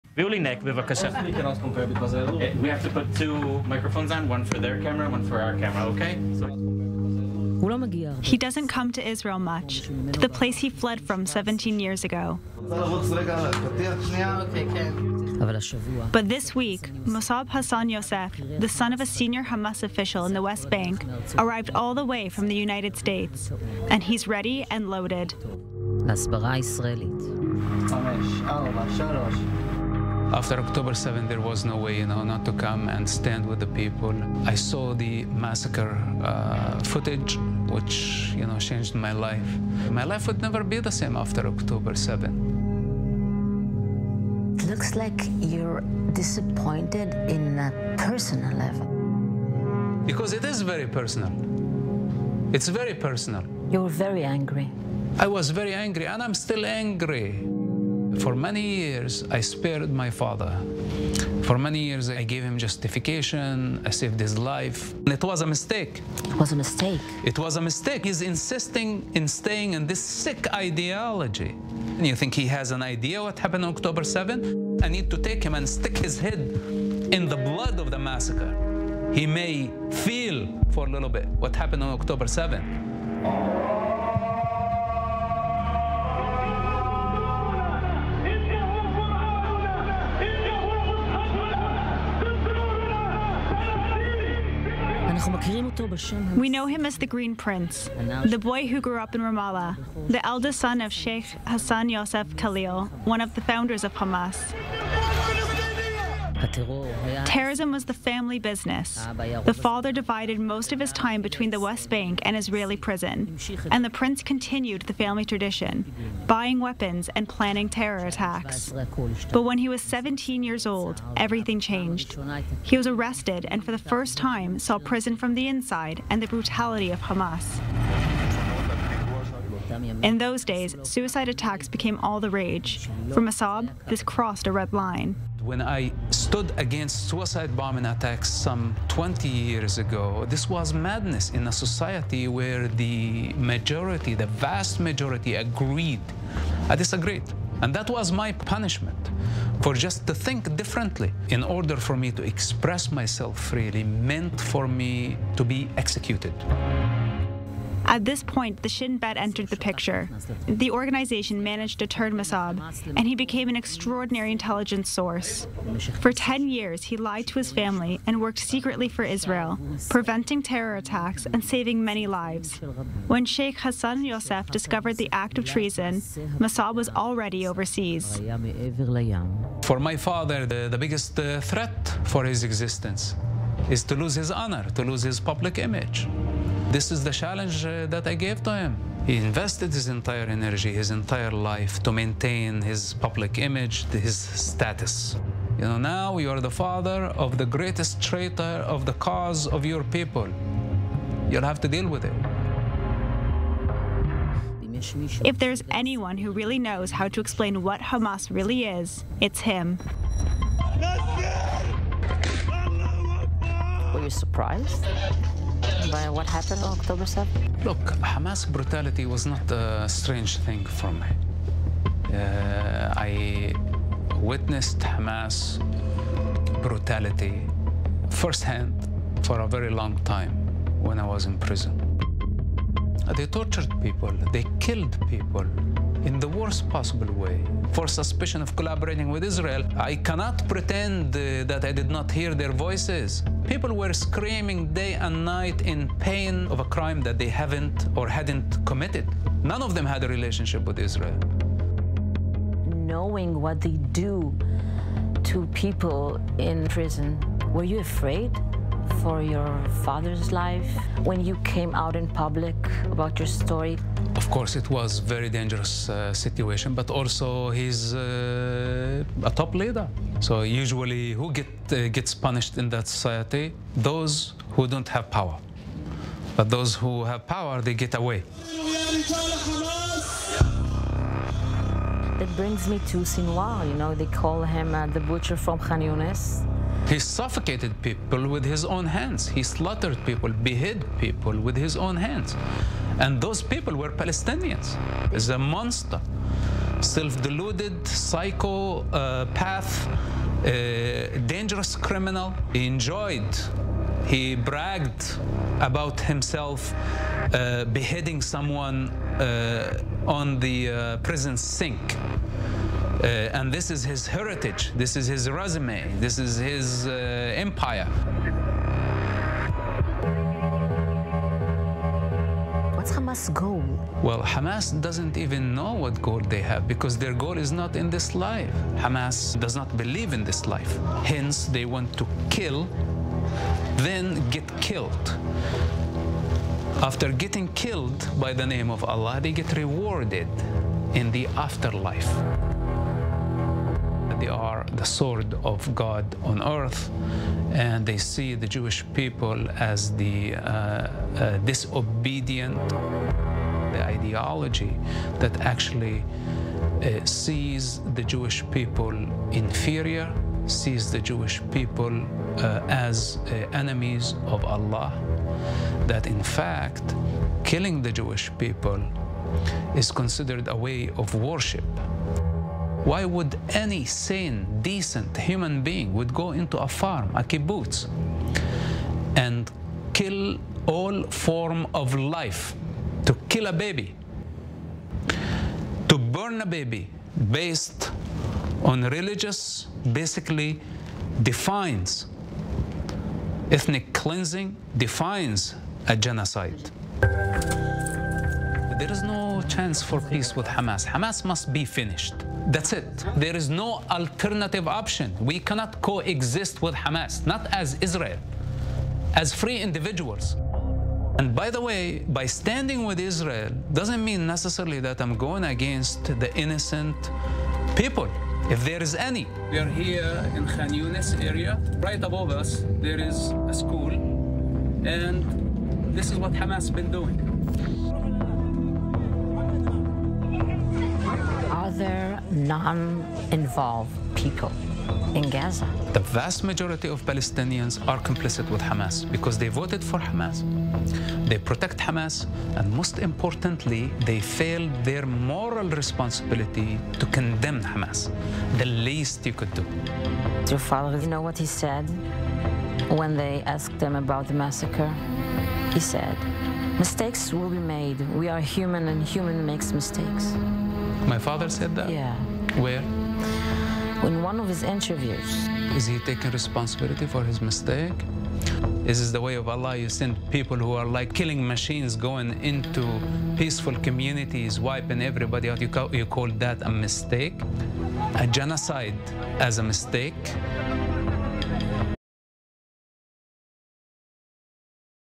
'Son of Hamas' Mosab Hassan Yousef condemns Oct. 7 massacre
Mosab Hassan Yousef, the son of a senior Hamas leader, slams the terrorist organization's brutal massacre of Israelis on October 7.

Adapted from Channel 12.